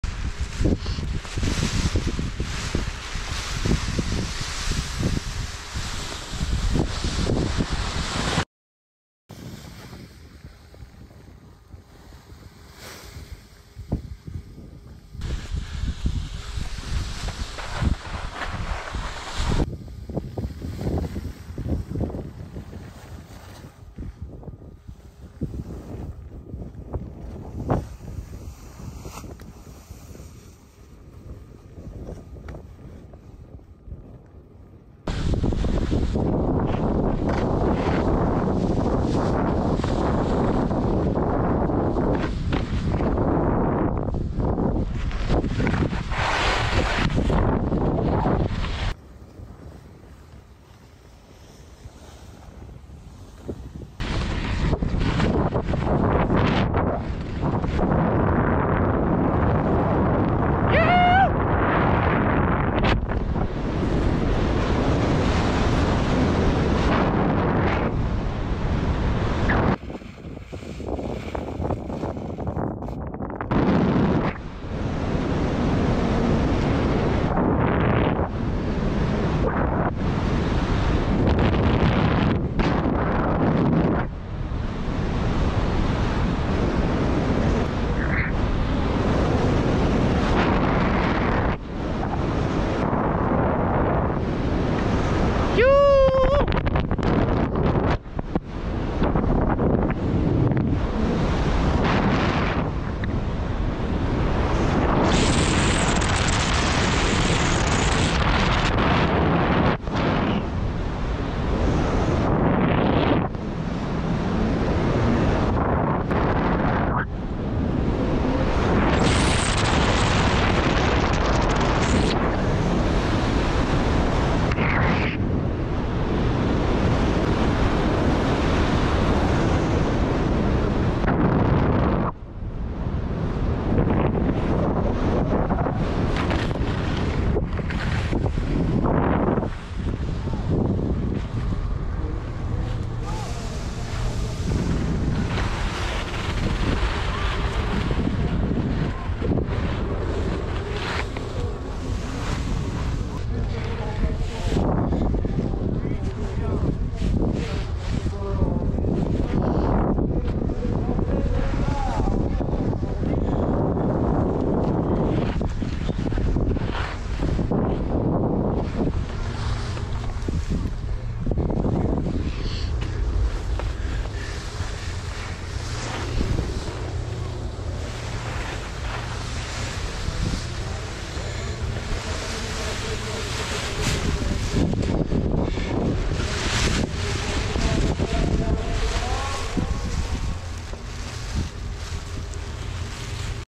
Paragliding ski launch Alpe d’Huez
Start at Signal in Northwest direction. After takeoff turning back right to land in Alpe d’Huez. Short flight but ideal to learn and hone your ski fly skills!